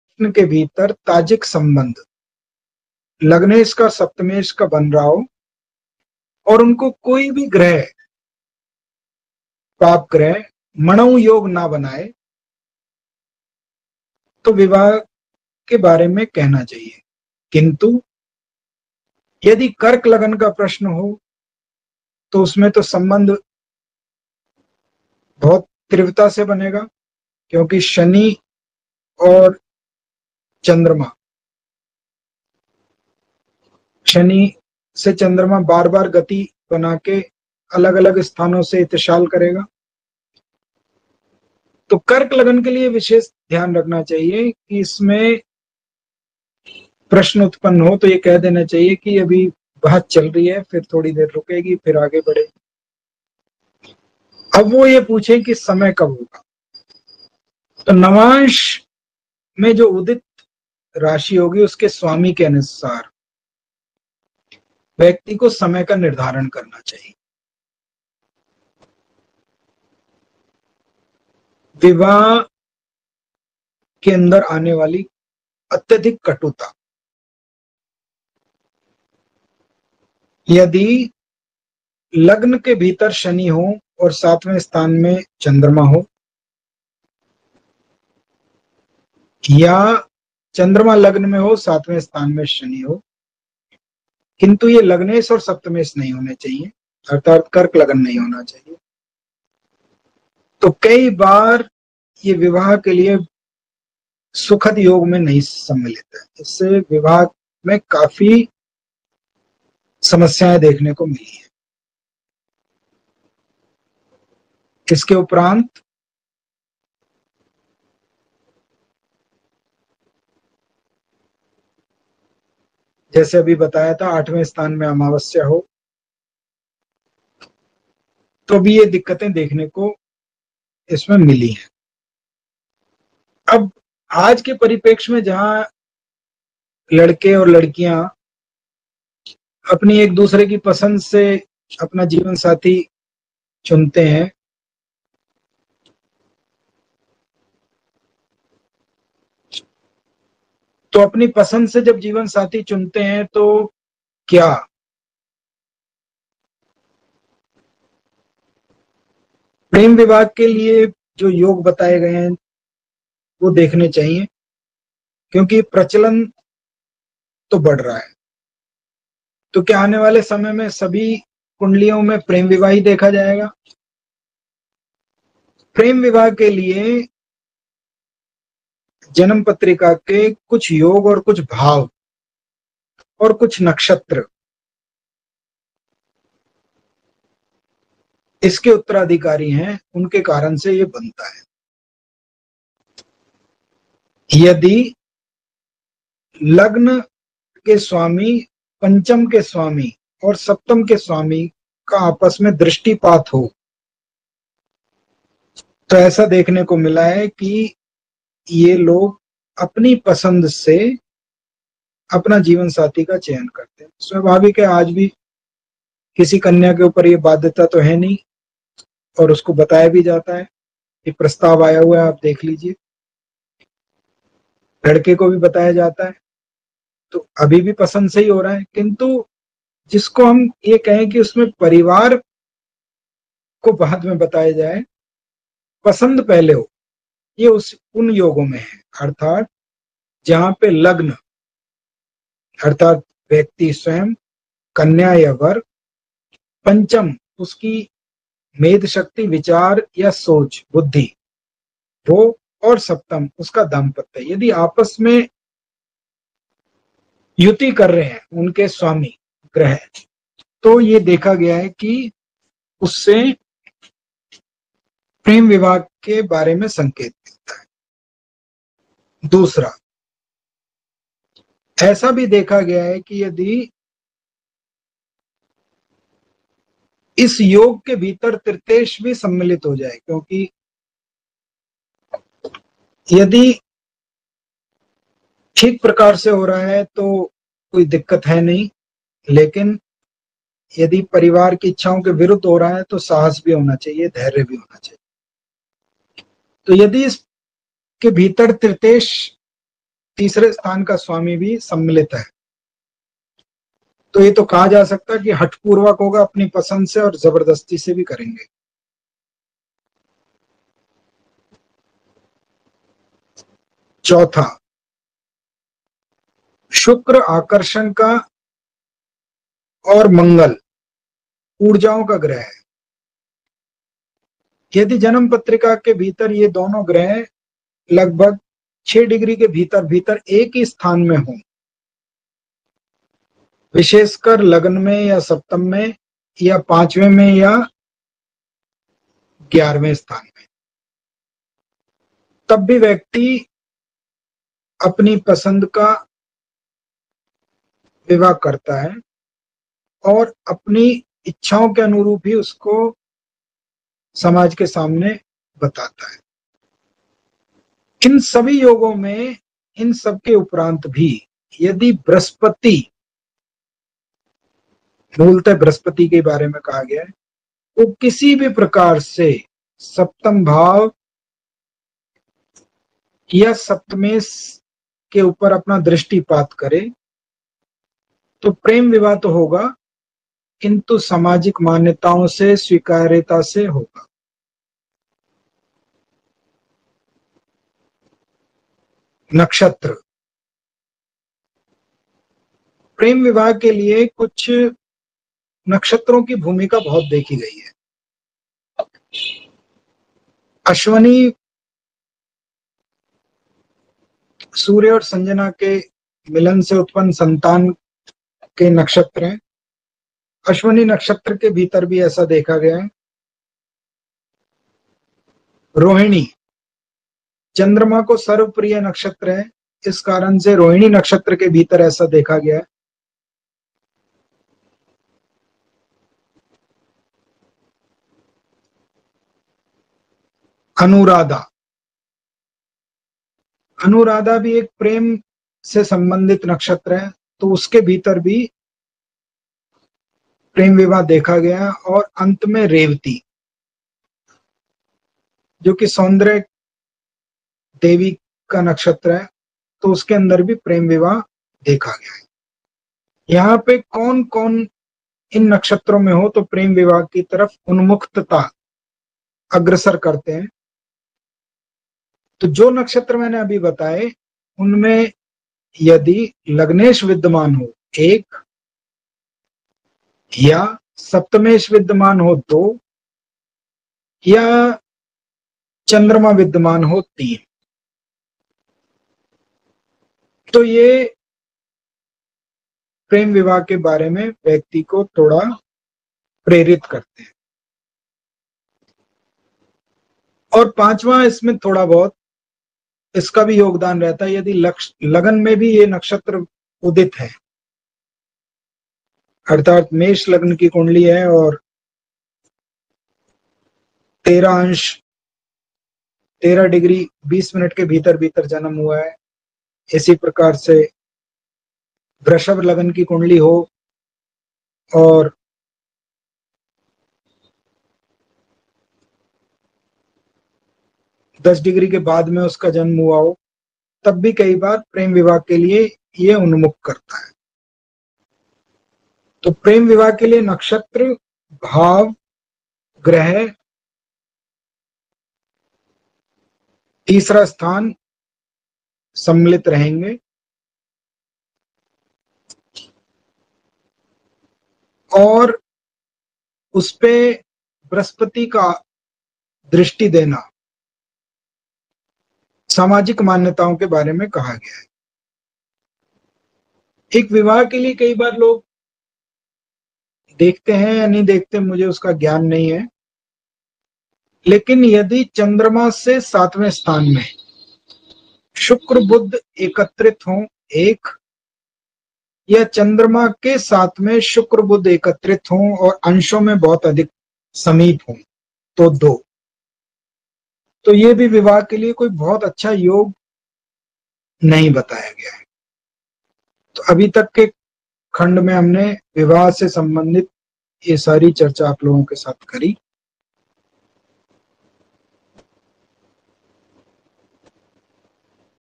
विवाहवृंदावनम श्रंखला खंड 5 प्रेमविवाह विचार marriage series part 5
Astrological discussion about marriage
here we start a series about marriage and matching of Boy and Girl ( in respect of marriage match making  )
it not a matching only two person
it is also a matching of two families who bound with the same ideology to expand their family
according to vedic Astrology Jyotish, which factor should be considered in this process
it's the 5th part of the series here. we covered the astrological combination of love marriage